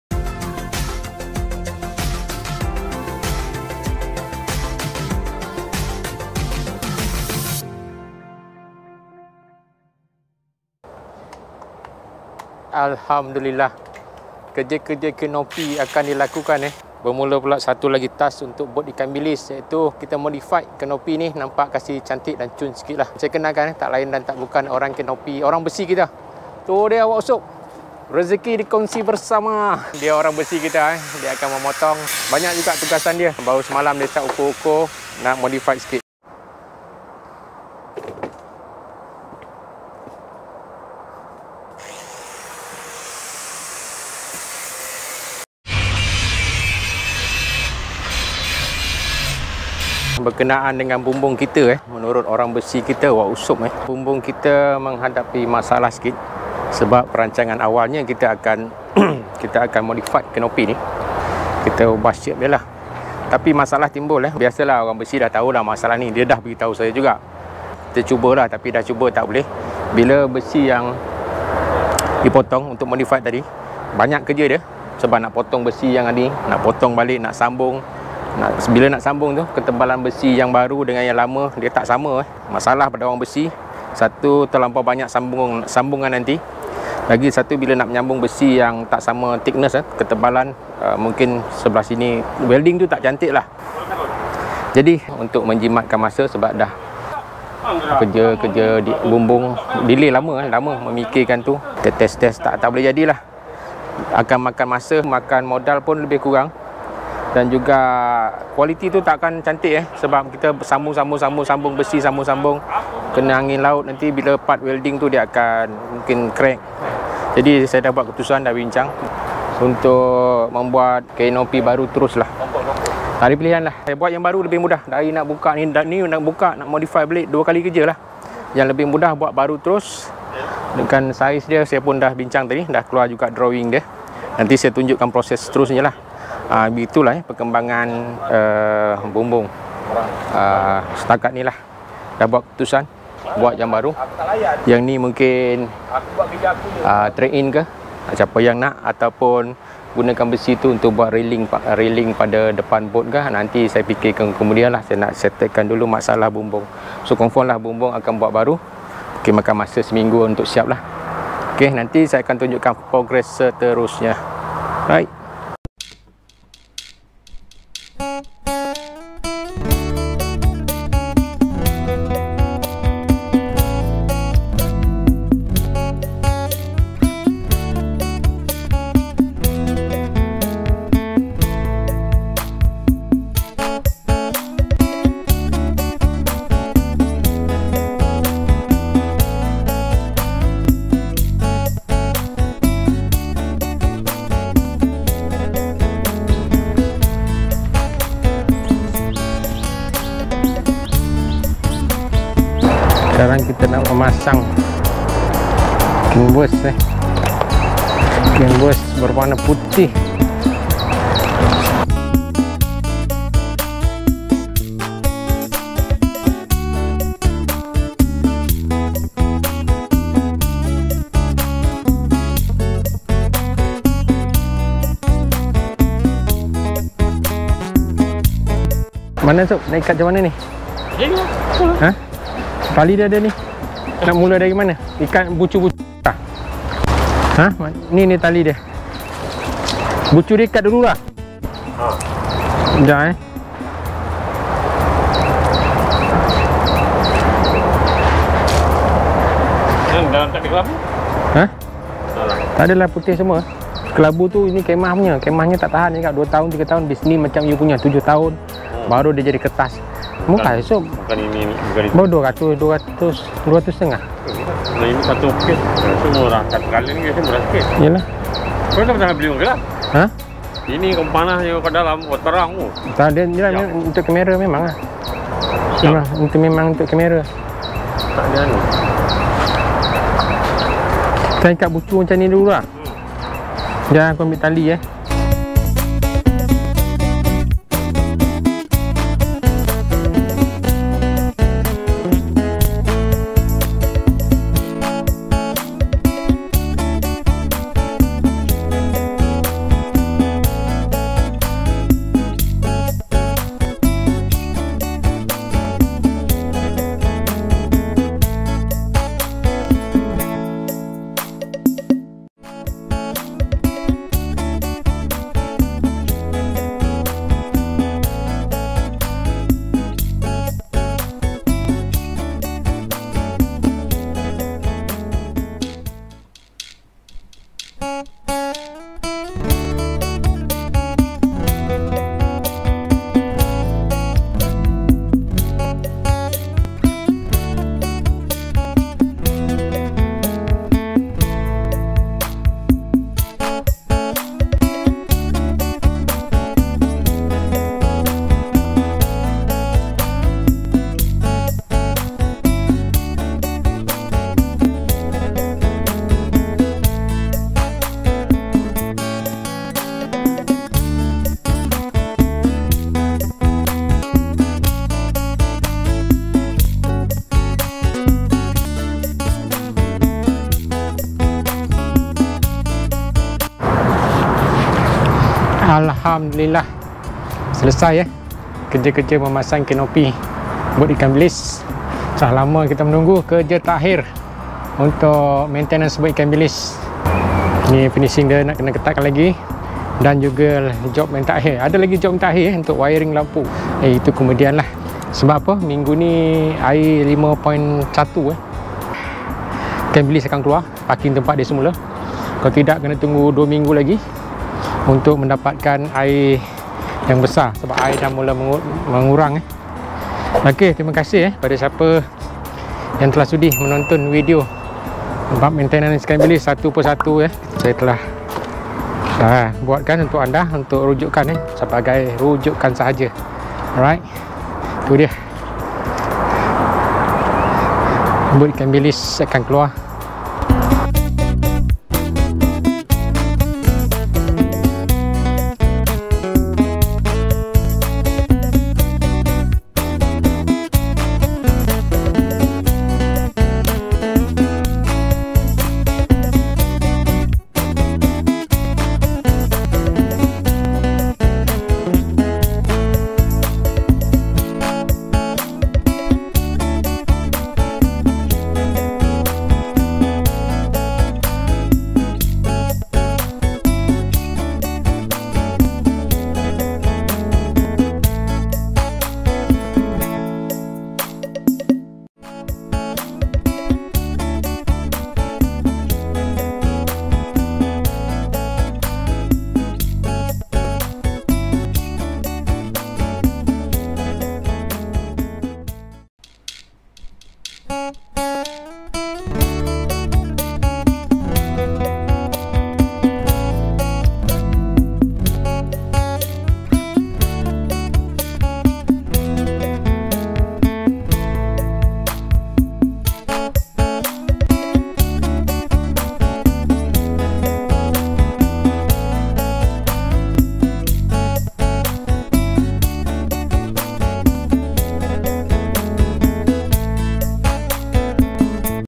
SSIB #20 - CANOPY & CANVAS
Rindu Abang dengan penonton semua.. maafkan Abang sebab video kali ni lama baru nak upload.

Okay, sekarang bot Abang dah 90% siap, canopy dan canvas pun dah cun. Tinggal buat wiring lampu jer lagi, lepas tu Abang dah boleh gi mancing, gigil tangan dah ni..

Saksikanlah serba sedikit proses memasang canopy & canvas untuk bot ikan bilis ya.  Maaf, ada bahagian yang audio kurang memuaskan.

Selamat menonton dan salam sayang dari Abang,
Azman Ikan Bilis